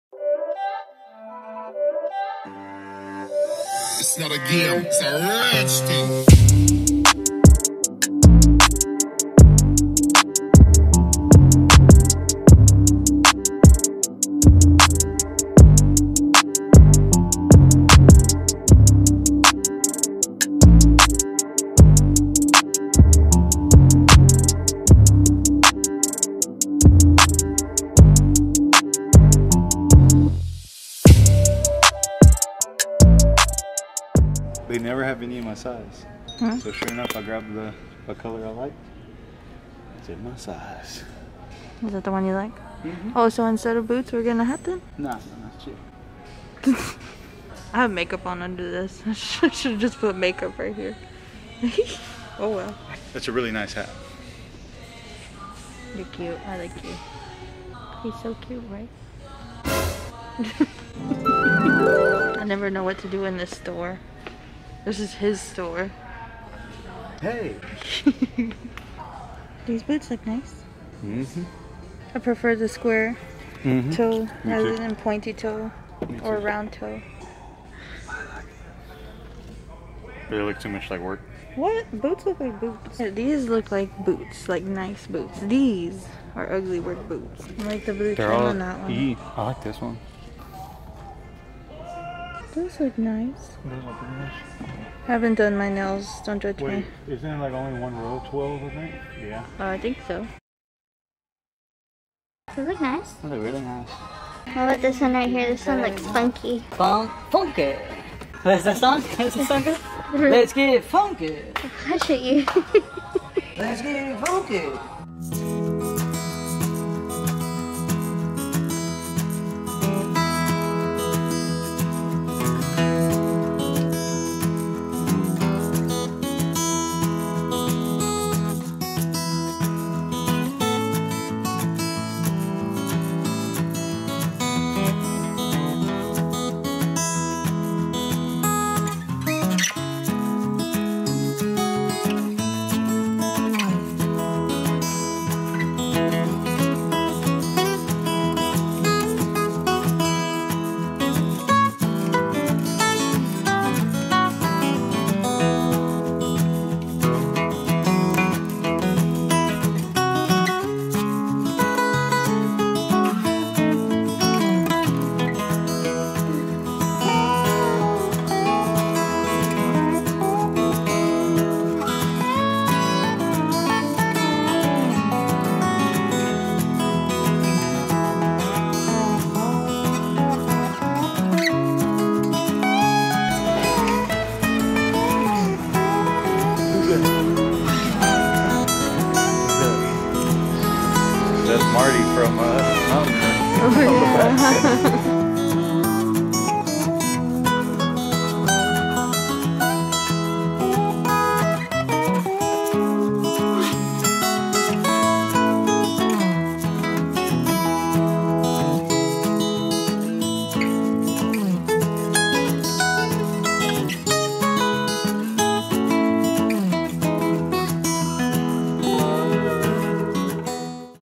BOOTS. BEARS. BATTLESTAR GALACTICA.
Today we are on the hunt for some boots and guess what? We ended up with none! Lol maybe next time! also we call each other bear 💖

DOWN HERE!!

If you made it all the way down here comment: BOOTS BICH